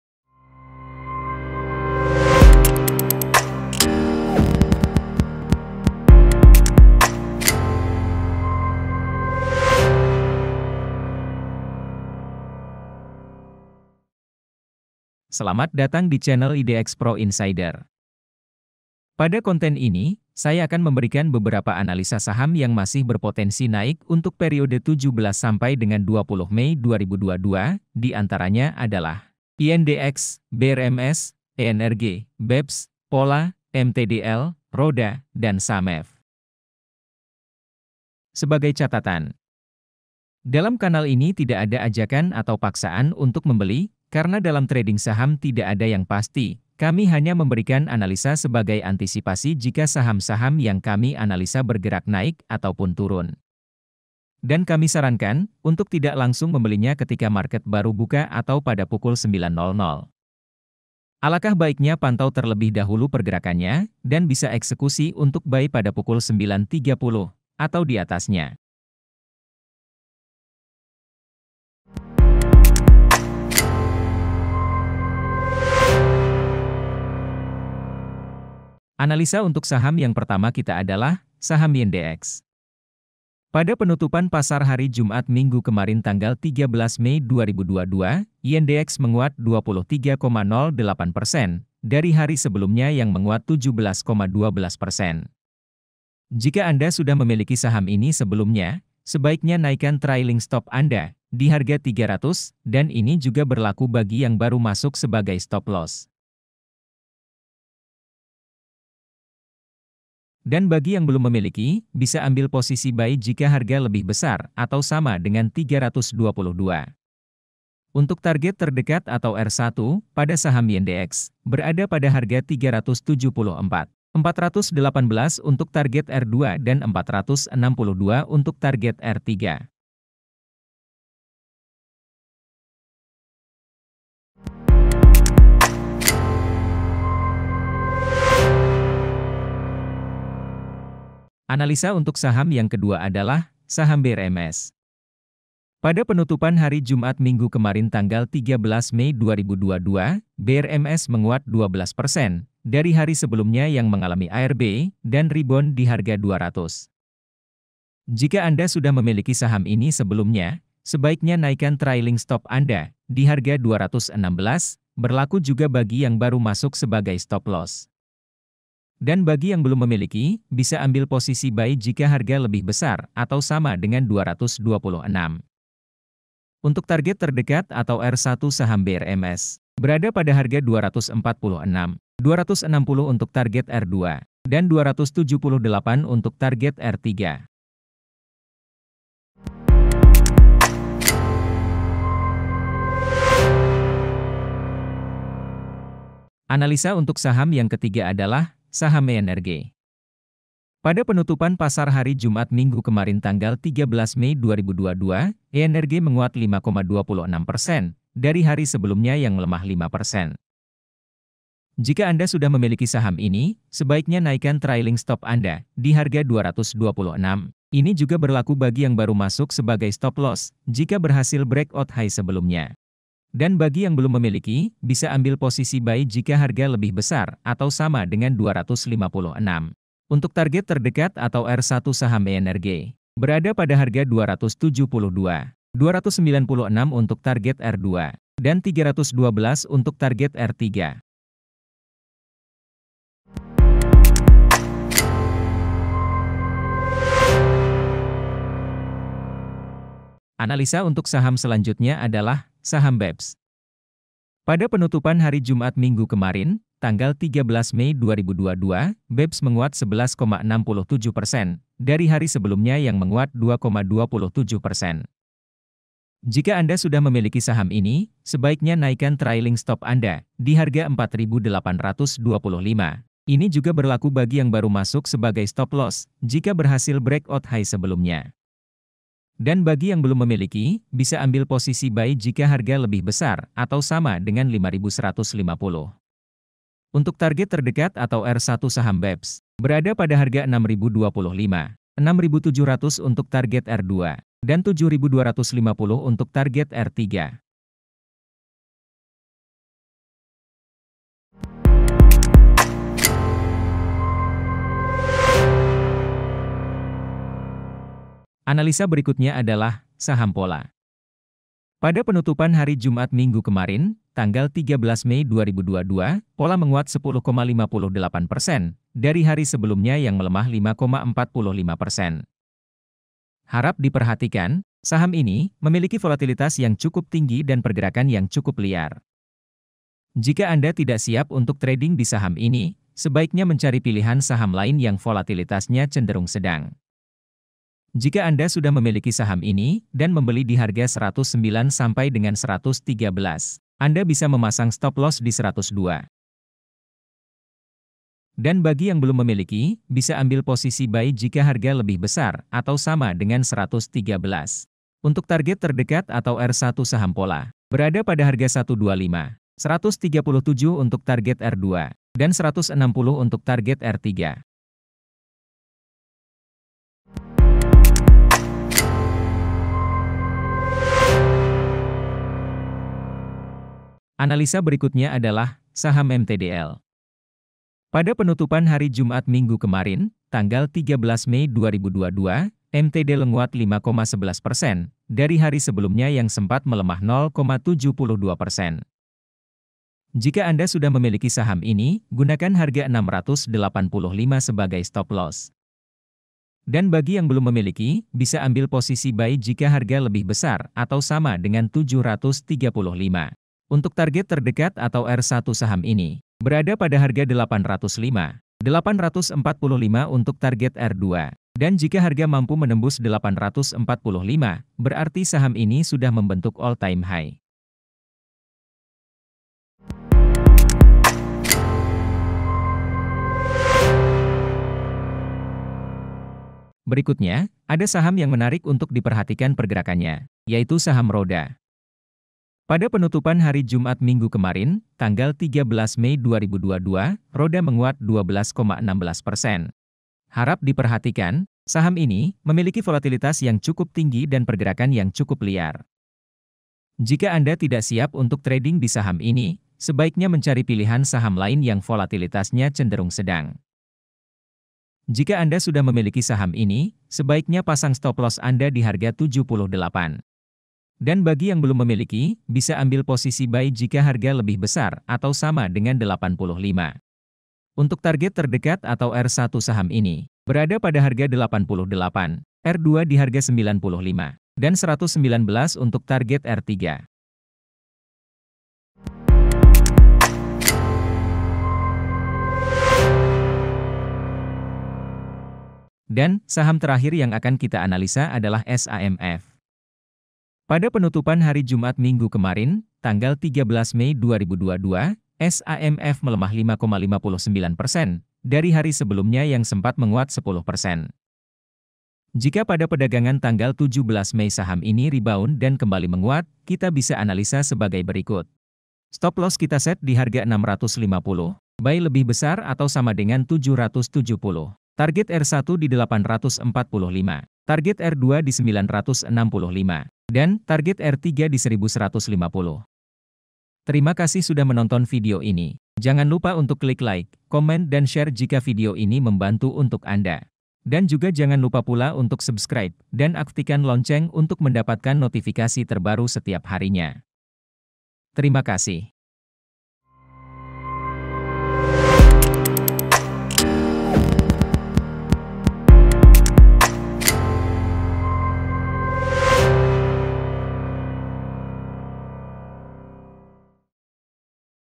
#1 Analisa Saham INDX BRMS ENRG BEBS POLA MTDL RODA SAMF periode 17 sampai dengan 20 Mei 2022
Analisa saham pada konten ini bertujuan untuk membantu rekan rekan Trader atau Investor dalam memilih saham yang akan dibeli, kami tidak pernah Mengajak atau memaksakan rekan rekan semua untuk membeli saham2 yang kami Analisa.
semua video ini dapat menbantu.

Terima kasih